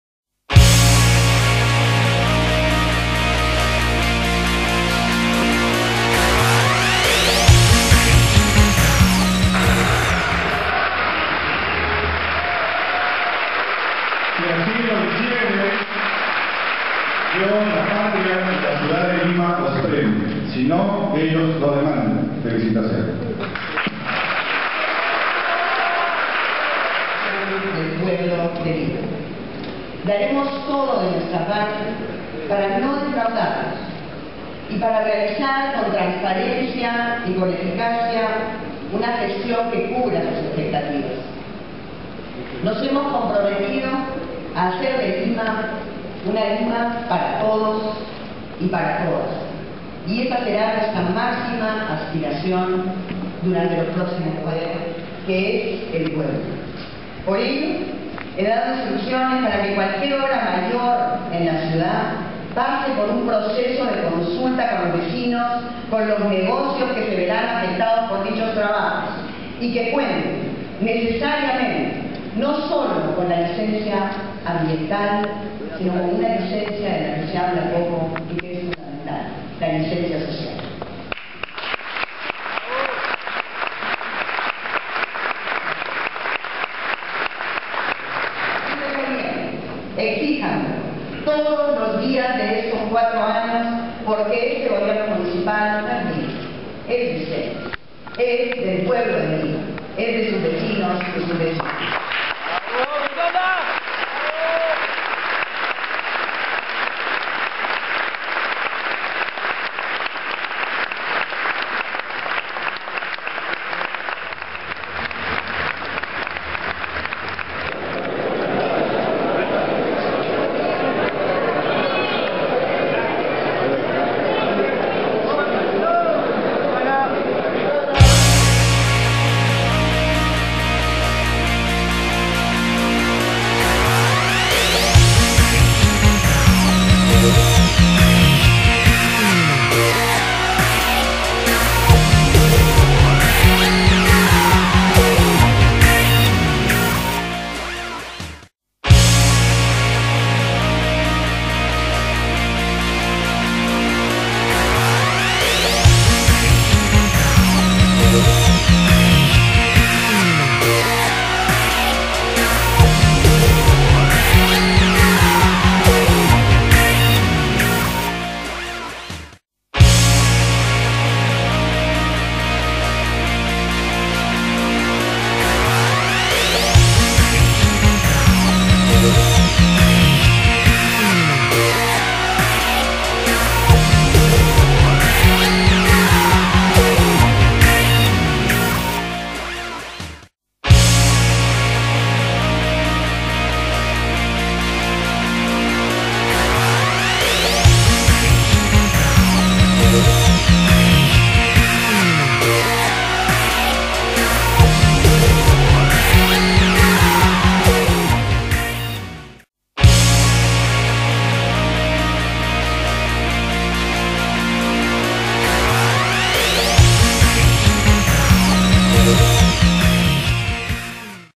Ceremonia de Juramentación del Consejo Metropolitano de Lima 2011 - 2014
Estas son unas cuantas imágenes de la Ceremonia de Juramentacion de las nuevas autoridades en nuestra ciudad capital, la misma que contó con la presencia del Presidente de la República.
Así mismo nuestra alcaldesa, Susana Villaran, señaló que los cinco ejes de su gestión serán: la concertación, la eficiencia, el principio de autoridad, la inclusión y la transparencia.